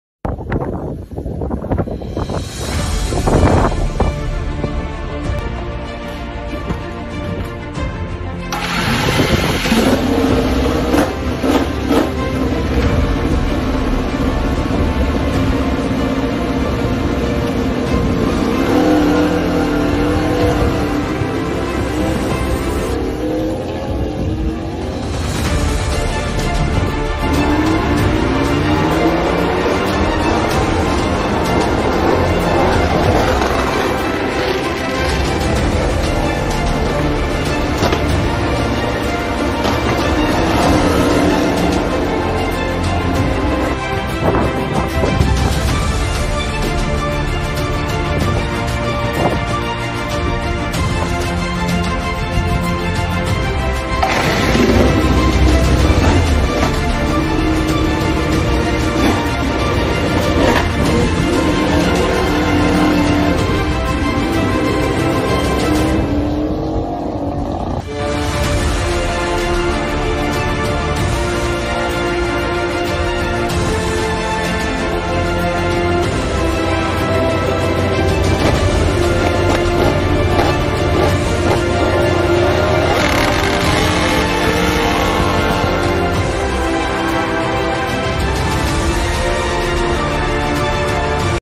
Ducati Team‼️MotoGP Qatar Test
Ini adalah video test pra musim tim Ducati di sirkuit  Losail Qatar pada tahun 2021 yang di ikuti oleh beberapa pembalap andalan tim Ducati, yaitu:

1. Michele Pirro (Tes Rider Tim Ducati Corse)
2. Jack Miller (Rider Ducati Lenovo)
3. Jorge Martin (Rider Pramac Ducati)